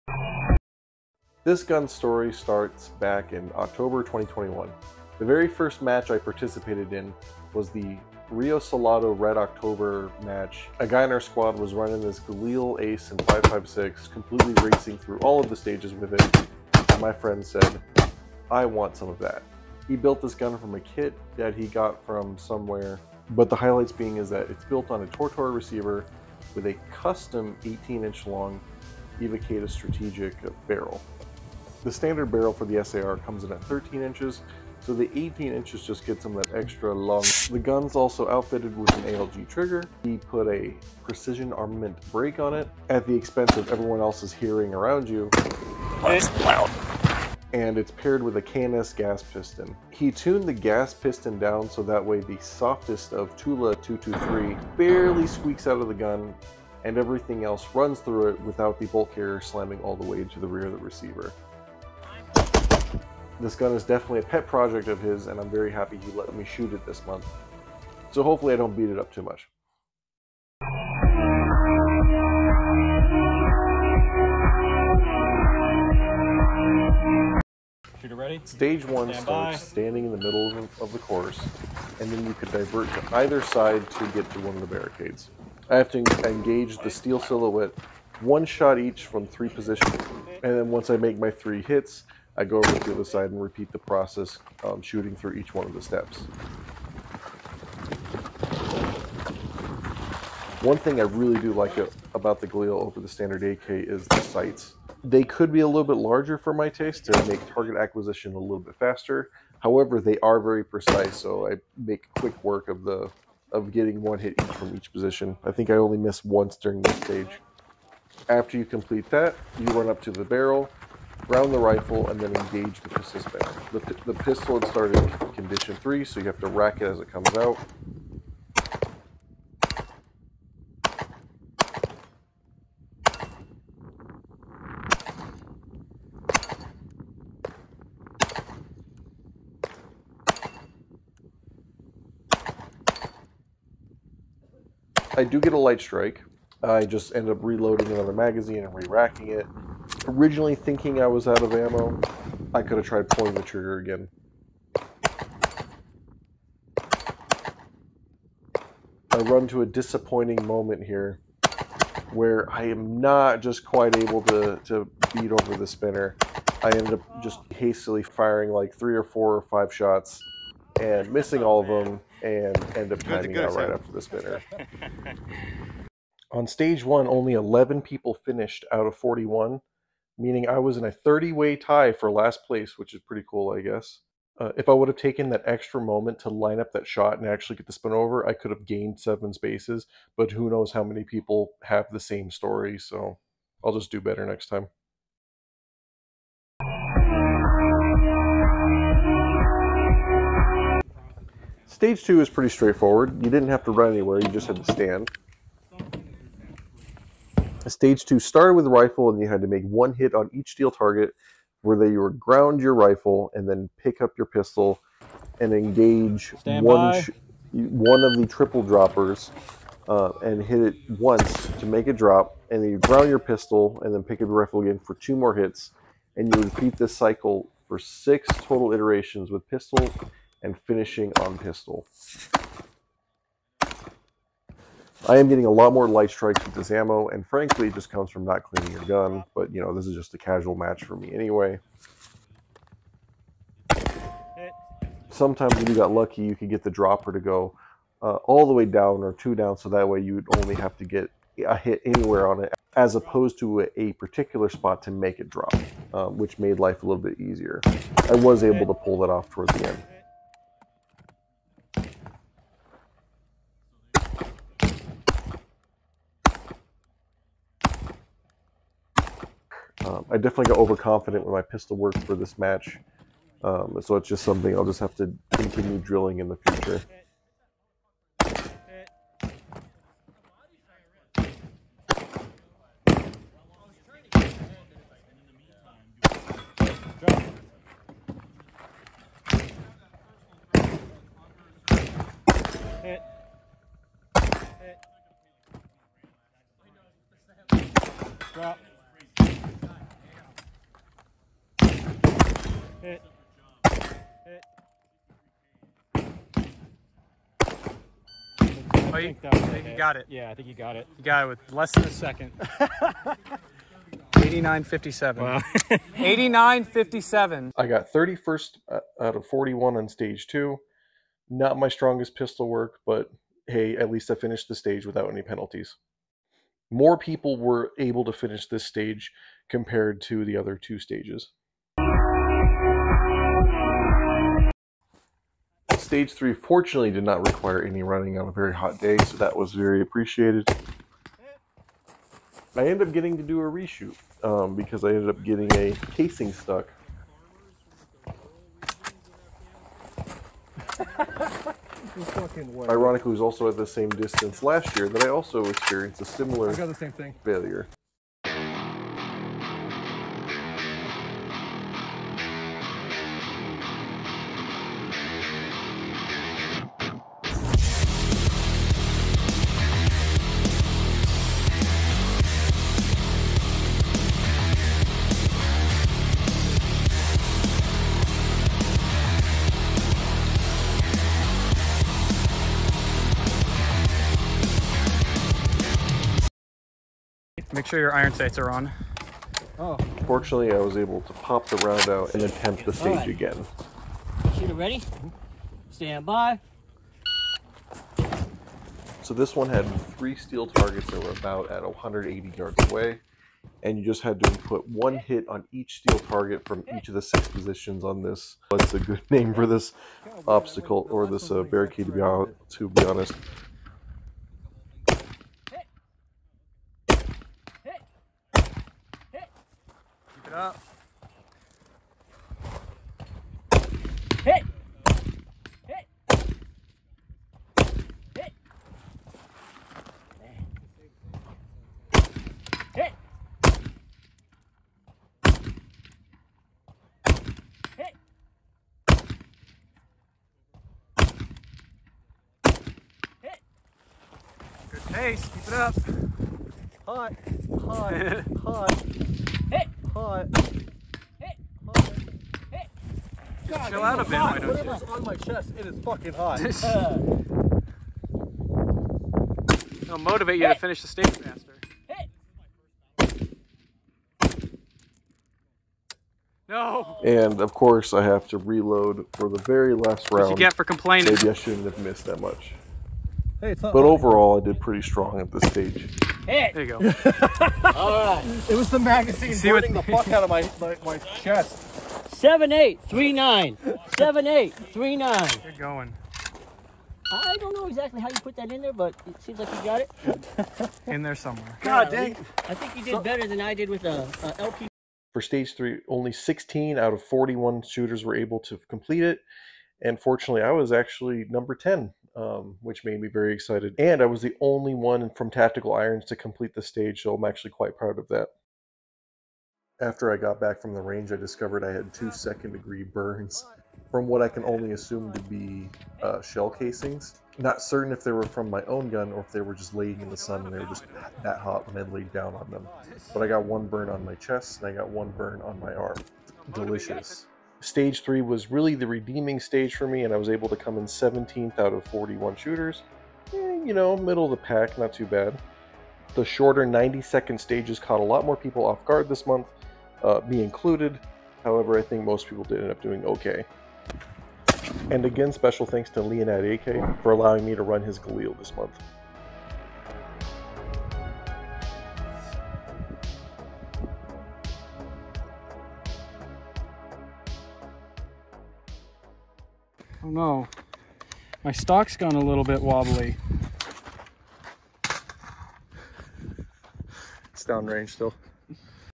2 Gun Action Challenge Match - Pima Pistol Club 7/15/2023 - @LeinadAK's Galil SAR
This month I am borrowing @LeinadAK's Galil SAR! Special thanks to him and @henrysmithtv!

Junk I'm using:
Galil SAR built on a
Tortort reciever with Evocatus Strategic barrel
ALG AKT-EL
KNS Precision Gas Piston
Precision Armament M4-72 Severe Duty Compensator
Magpul Sling

CZ P-07
with Cajun Gunworks Pro Trigger Package
using an Orpaz P-07/P-09 Class I and II Retention Holster

Thank you @WhiteBatAudio for the music!

0:00 Intro to the Galil
1:18 Stage 1
3:27 Stage 2
5:44 Stage 3
8:43 Final Results